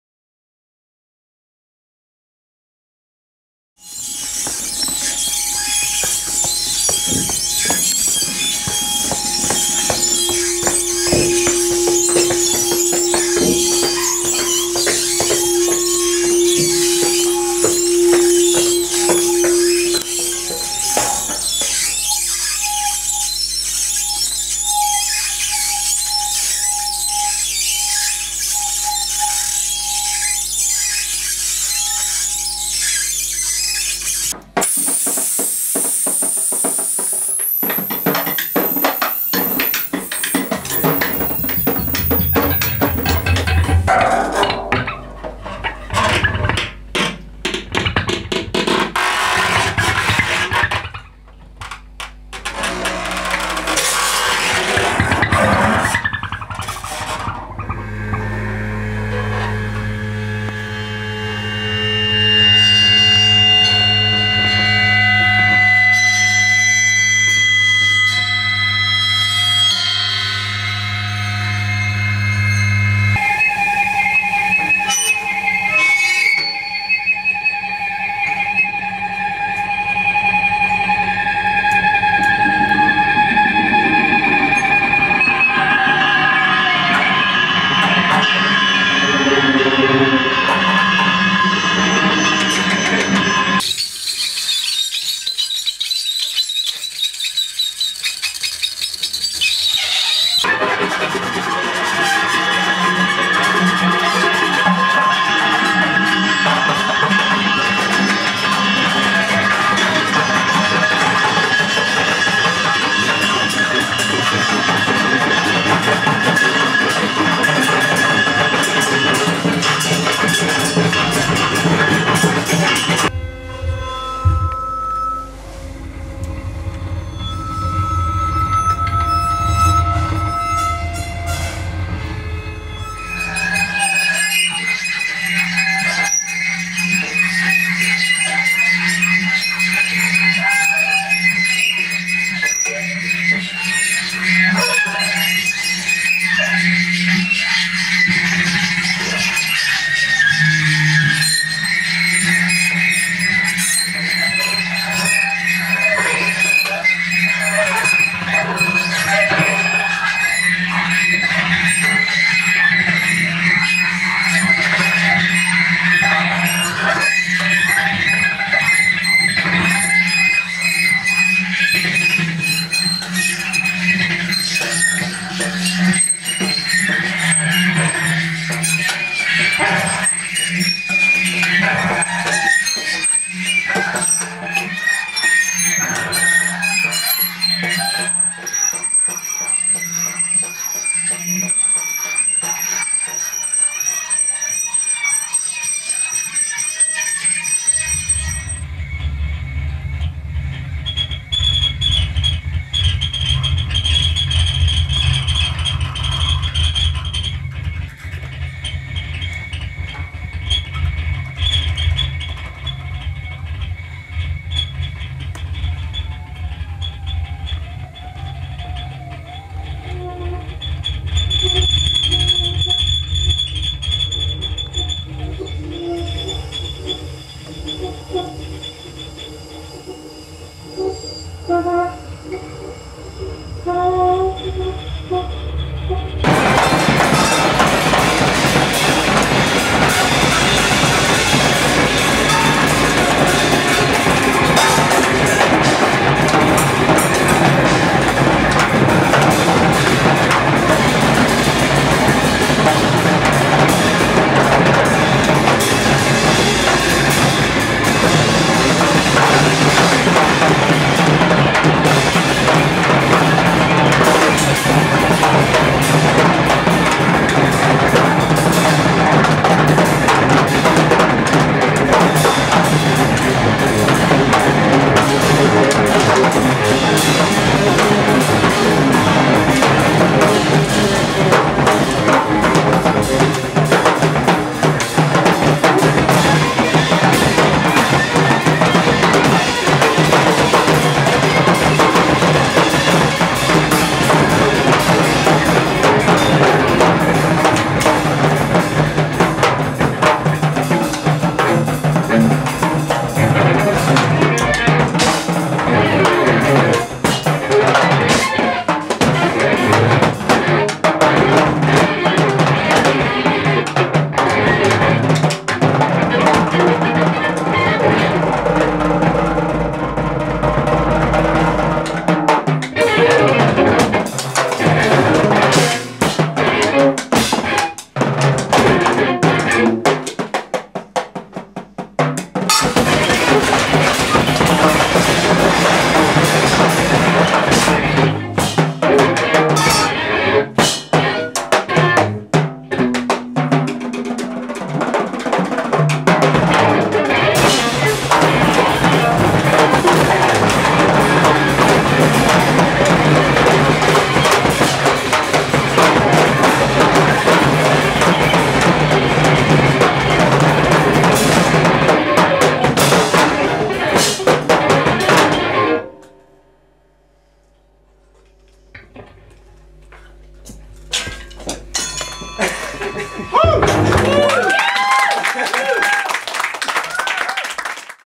RYAN JEWELL, PATRICK FARMER & DAVID THOMAS
RYAN JEWELL, PATRICK FARMER & DAVID THOMAS bring on a heap of acoustic squeals in THE HUNTER GRACCHUS's practice space...
this was a Singing Knives event.